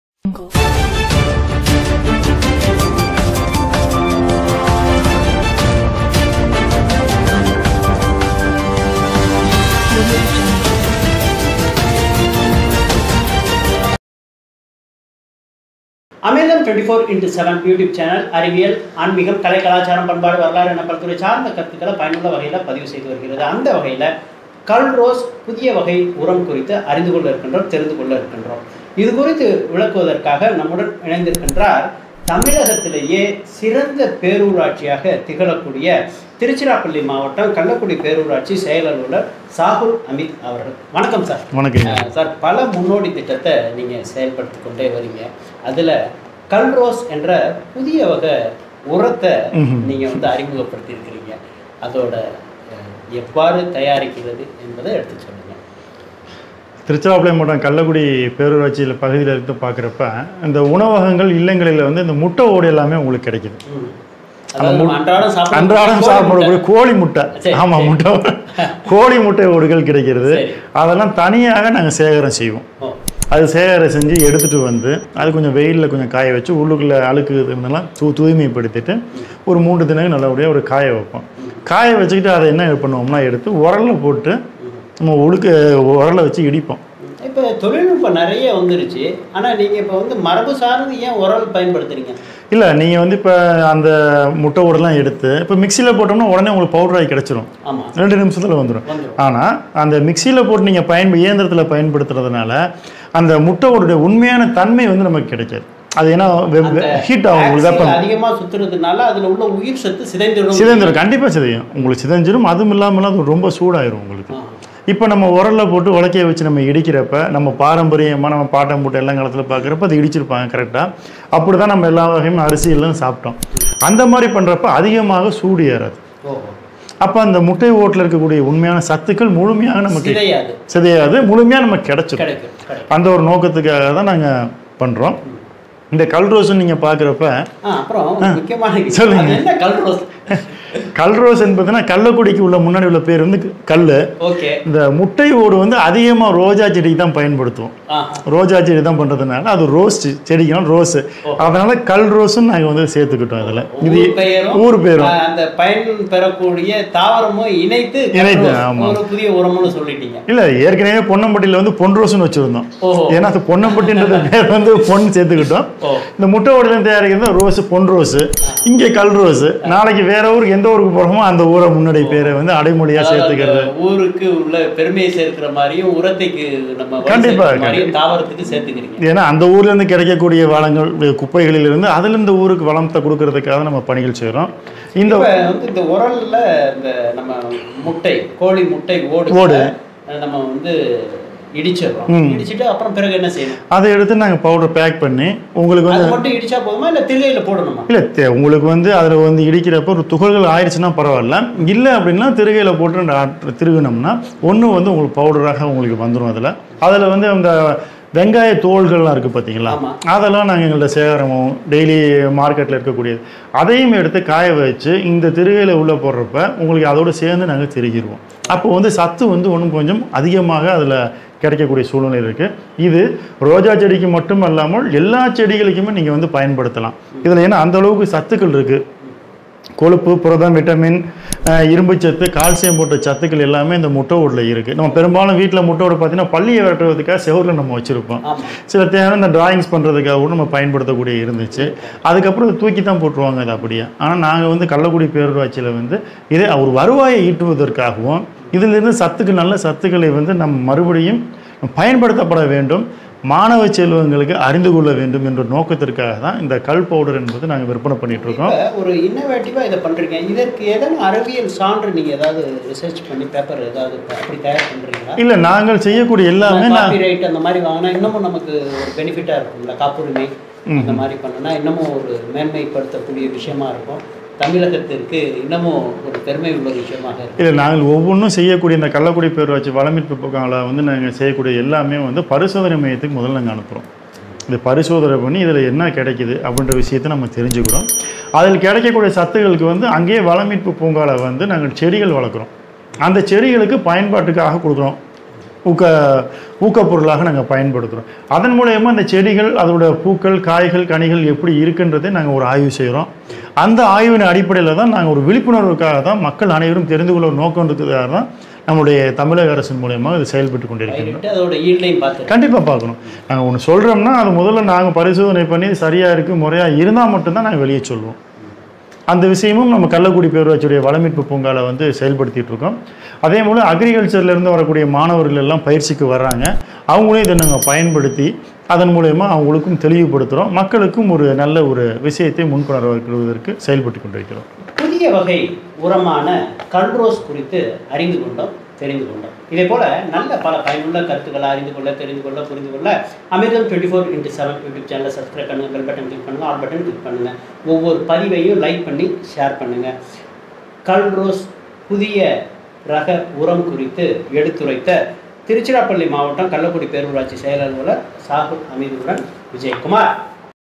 கல்ரோஸ் இயற்கை உரம் தயாரிக்கும் எளிய முறை.
கல்லக்குடி பேரூராட்சி செயல் அலுவலர் சாகுல் அமீது உடன் நேர்காணல்